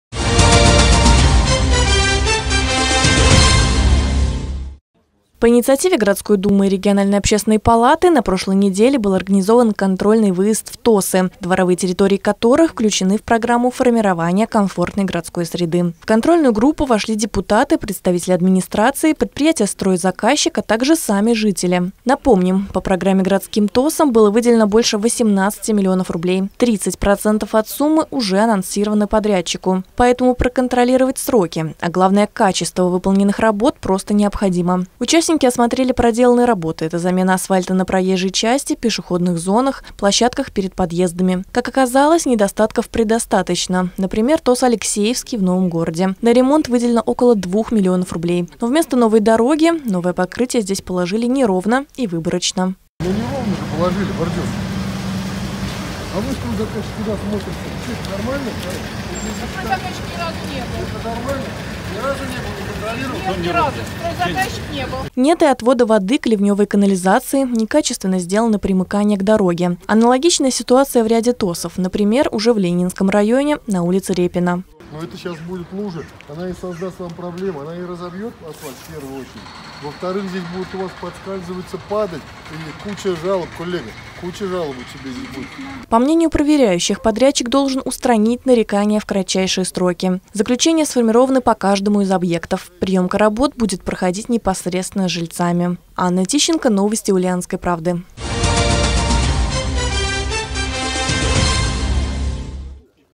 Благоустройство с лужами. В ульяновских дворах отремонтировали дороги с нарушениями
По инициативе Городской думы и региональной общественной палаты на прошлой неделе был организован контрольный выезд в территориальные общественные самоуправления, дворовые территории которых включены в программу «Формирование комфортной городской среды».